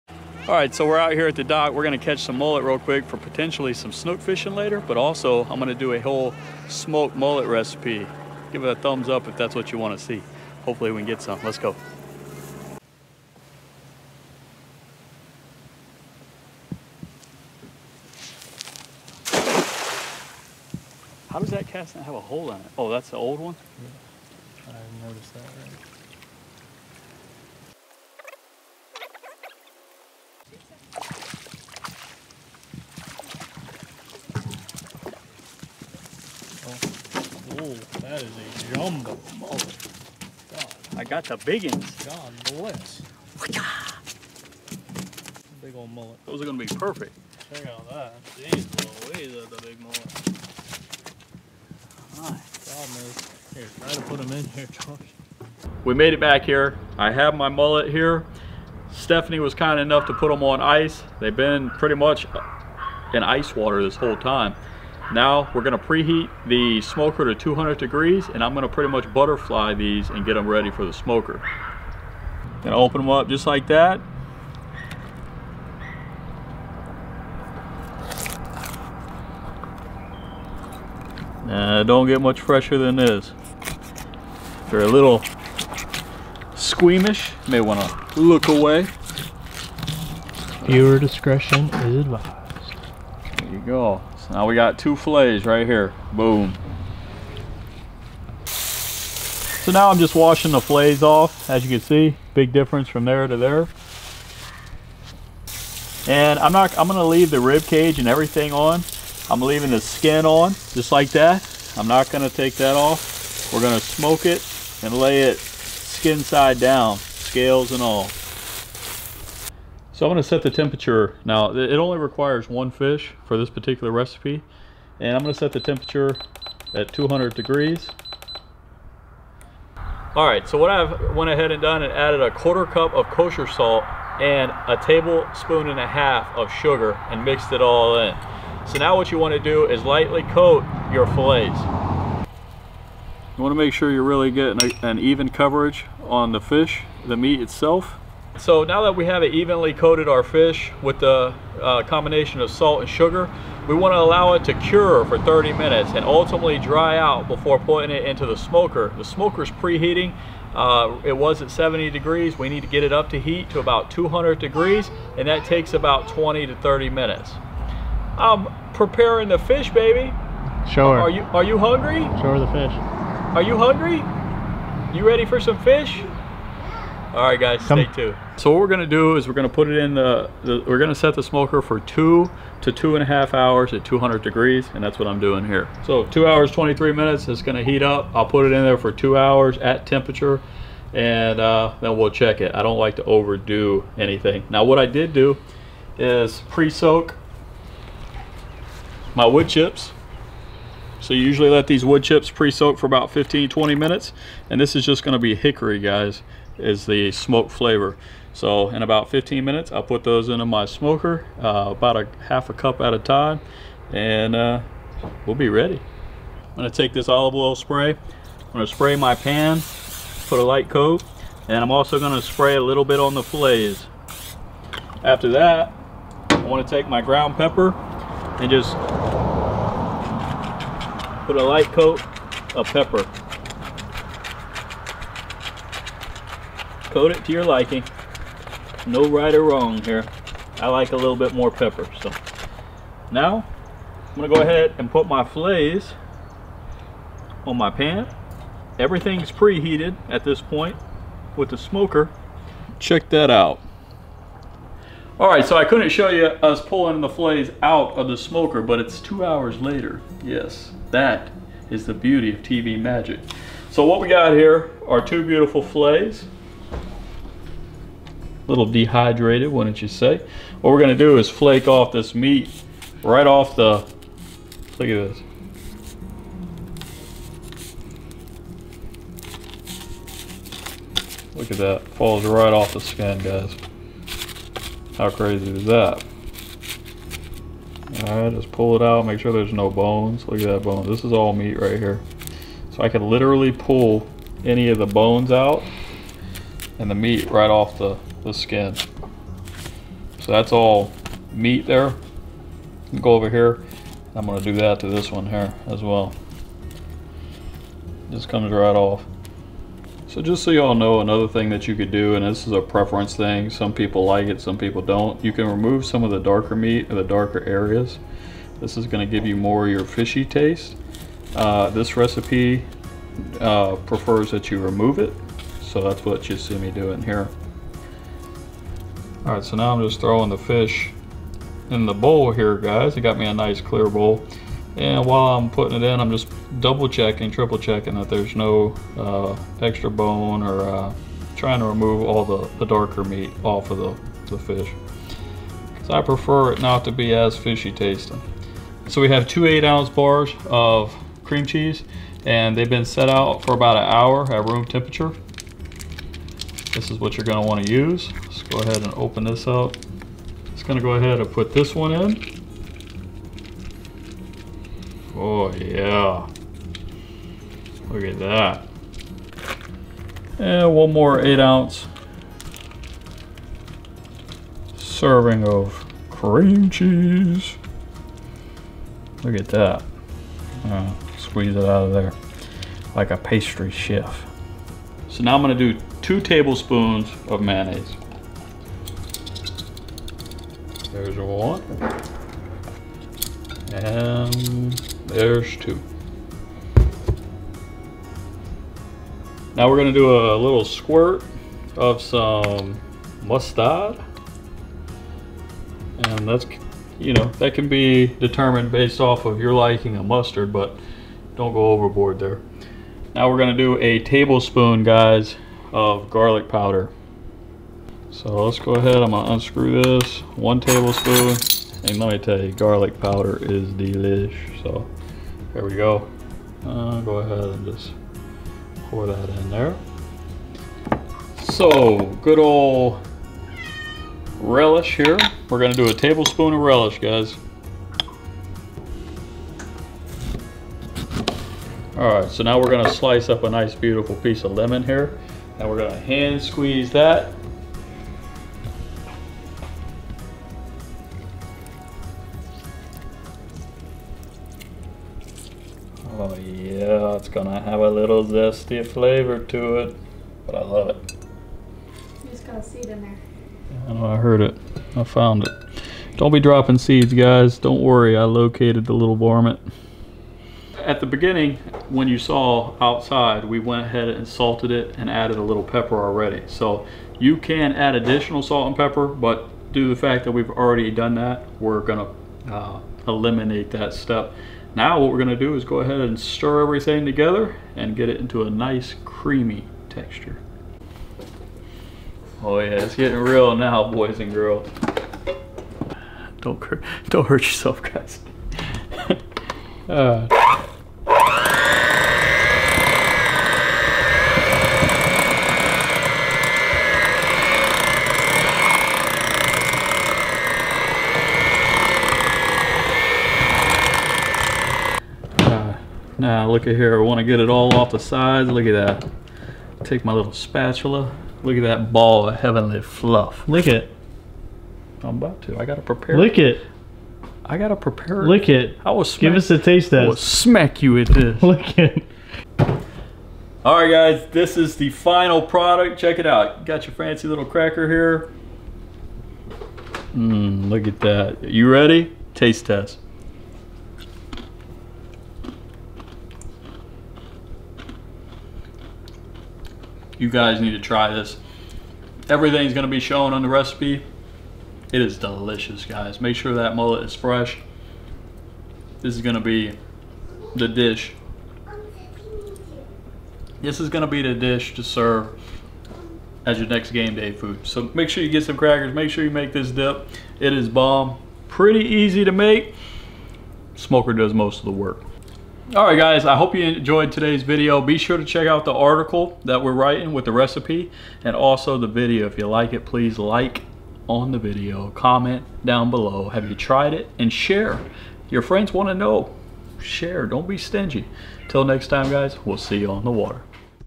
Mullet Catch & Cook - Best Smoked Mullet Dip
Mullet catch and cook, watch as we castnet mullet, and show you how to make the best-smoked mullet dip. This recipe is perfect for those who do not like the fishy taste typically consuming fish. With just a few ingredients, you can make this dish, and your friends will be asking how to make it and when can they have more.